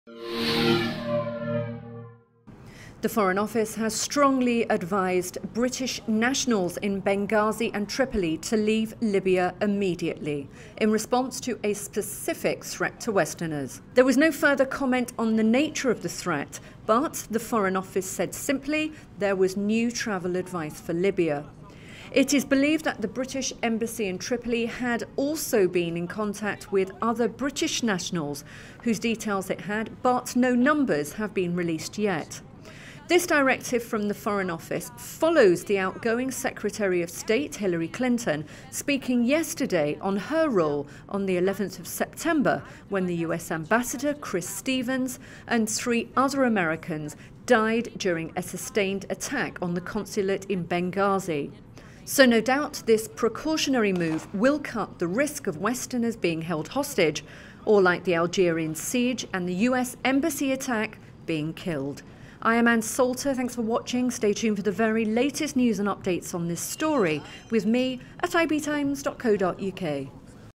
Foreign Office warns Britons to leave Libya's Benghazi
The foreign Office has strongly advised British nationals in Benghazi and Tripoli to leave Libya immediately, in response to a specific threat to Westerners.

There was no further comment on the nature of the threat, but the Foreign office said simply, there was new travel advice for Libya

It is believed that the British Embassy in Tripoli had also been in contact with British nationals whose details it had. But no numbers have been released.

This directive from the foreign office follows the outgoing Secretary of State Hilary Clinton speaking  yesterday on her role On 11 September, when the US ambassador Chris Stevens and three other Americans died during an attack on the consulate in Benghazi.

So no doubt this precautionary move will cut the risk of westerners being held hostage or like the Algerian siege and US Embassy attack, being killed.

Written and presented by Ann Salter.